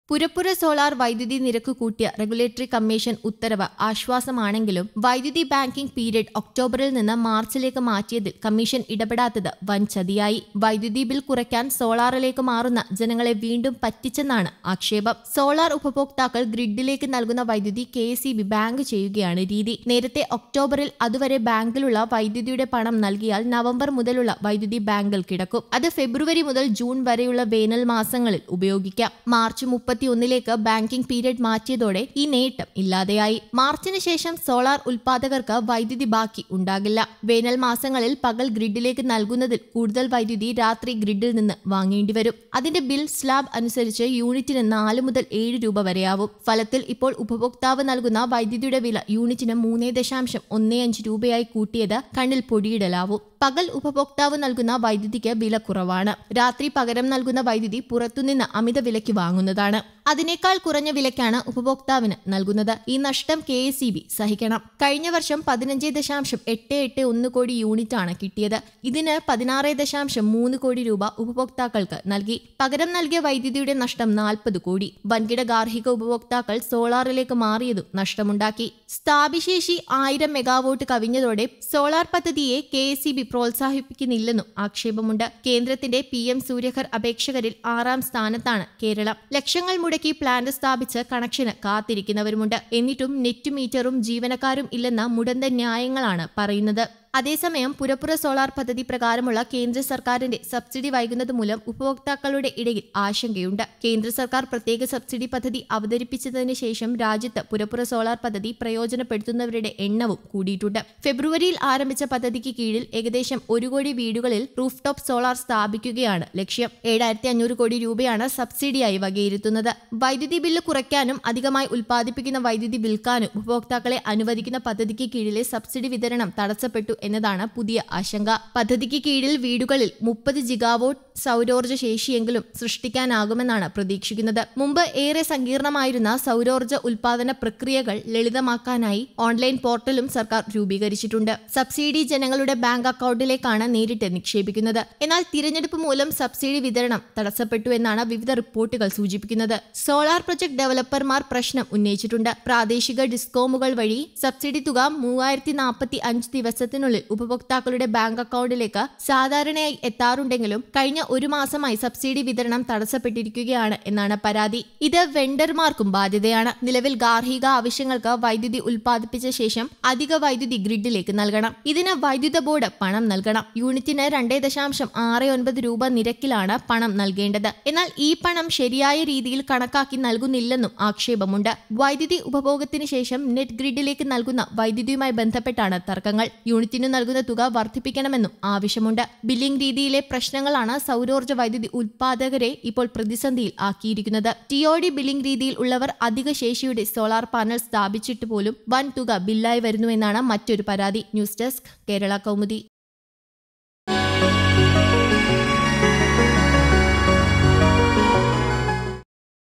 സോളാര്‍ എടുത്തവര്‍ പറ്റിക്കപ്പെട്ടു,ഇനി വൈദ്യുതി ബാക്കി ഉണ്ടാകില്ല | KSEB Solar | Kerala
പുരപ്പുറ സോളാര്‍ വൈദ്യുതി നിരക്ക് കൂട്ടിയ റെഗുലേറ്ററി കമ്മിഷന്‍ ഉത്തരവ് ആശ്വാസമാണെങ്കിലും വൈദ്യുതി ബാങ്കിംഗ് പീരിയഡ് ഒകേ്ടാബറില്‍ നിന്ന് മാര്‍ച്ചിലേക്ക് മാറ്റിയതില്‍ കമ്മിഷന്‍ ഇടപെടാത്തത് വന്‍ചതിയായി. വൈദ്യുതി ബില്‍ കുറയ്ക്കാന്‍ സോളാറിലേക്ക് മാറുന്ന ജനങ്ങളെ വീണ്ടും പറ്റിച്ചെന്നാണ് ആക്ഷേപം. സോളാര്‍ ഉപഭോക്താക്കള്‍ ഗ്രിഡിലേക്ക് നല്‍കുന്ന വൈദ്യുതി കെ.എസ്.ഇ.ബി ബാങ്ക് ചെയ്യുകയാണ് രീതി. നേരത്തേ ഒകേ്ടാബറില്‍ അതുവരെ ബാങ്കിലുള്ള വൈദ്യുതിയുടെ പണം നല്‍കിയാല്‍ നവംബര്‍ മുതലുള്ള വൈദ്യുതി ബാങ്കില്‍ കിടക്കും. അത് ഫെബ്രുവരി മുതല്‍ ജൂണ്‍ വരെയുള്ള വേനല്‍ മാസങ്ങളില്‍ ഉപയോഗിക്കാം. മാര്‍ച്ച് 31ലേക്ക് ബാങ്കിംഗ് പീരീഡ് മാറ്റിയതോടെ ഈ നേട്ടം ഇല്ലാതായി. മാര്‍ച്ചിന് ശേഷം സോളാര്‍ ഉല്‍പാദകര്‍ക്ക് വൈദ്യുതി ബാക്കി ഉണ്ടാകില്ല. വേനല്‍ മാസങ്ങളില്‍ പകല്‍ ഗ്രിഡിലേക്ക് നല്‍കുന്നതില്‍ കൂടുതല്‍ വൈദ്യുതി രാത്രി ഗ്രിഡില്‍ നിന്ന് വാങ്ങേണ്ടിവരും. അതിന്റെ ബില്ല് സ്ലാബ് അനുസരിച്ച് യൂണിറ്റിന് നാല് മുതല്‍ ഏഴ് രൂപ വരെയാവും. ഫലത്തില്‍ ഇപ്പോള്‍ ഉപഭോക്താവ് നല്‍കുന്ന വൈദ്യുതിയുടെ വില യൂണിറ്റിന് 3.15രൂപയായി കൂട്ടിയത് കണ്ണില്‍ പൊടിയിടലാവും. പകല്‍ ഉപഭോക്താവ് നല്‍കുന്ന വൈദ്യുതിക്ക് വില കുറവാണ്. രാത്രി പകരം നല്‍കുന്ന വൈദ്യുതി പുറത്തു നിന്ന് അമിത വിലയ്ക്ക് വാങ്ങുന്നതാണ്. അതിനേക്കാള്‍ കുറഞ്ഞ വിലയ്ക്കാണ് ഉപഭോക്താവിന് നല്‍കുന്നത്. ഈ നഷ്ടം കെ.എസ്.ഇ.ബി.സഹിക്കണം. കഴിഞ്ഞ വര്‍ഷം 15.881കോടി യൂണിറ്റാണ് കിട്ടിയത്. ഇതിന് 16.30കോടി രൂപ ഉപഭോക്താക്കള്‍ക്ക് നല്‍കി.